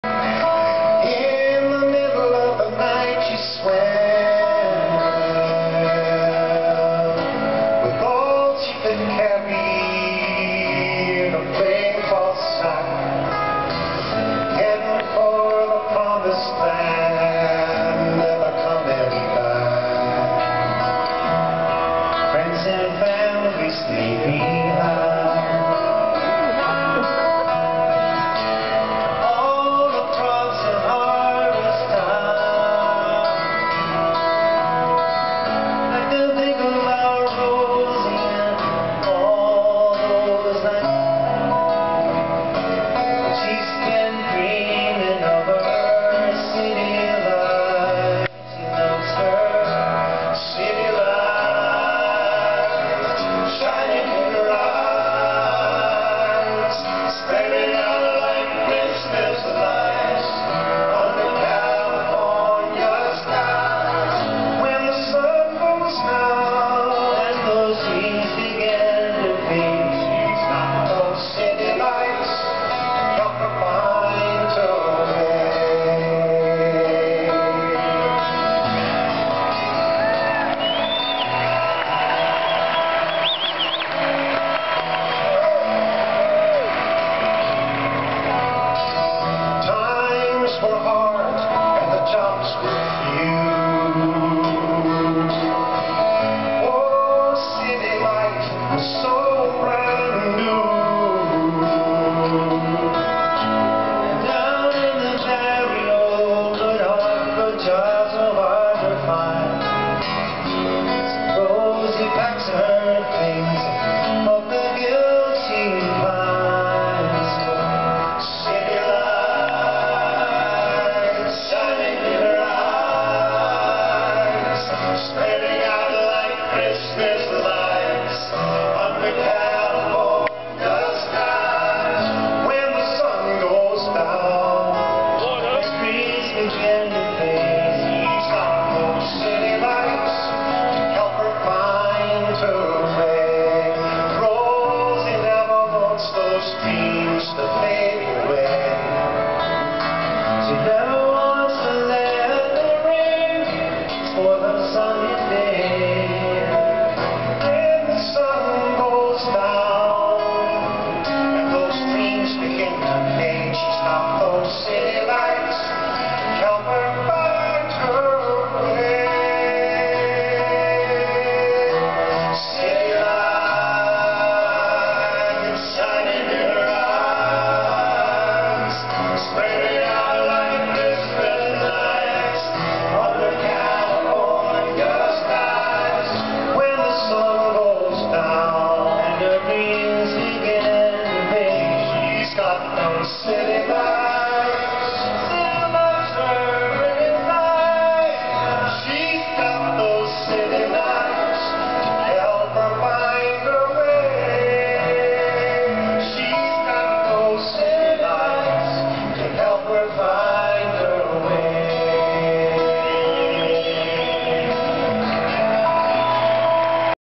James Taylor playing with Livingston Taylor
@ PNC Art Center Holmdel, N.J. June 23rd 2012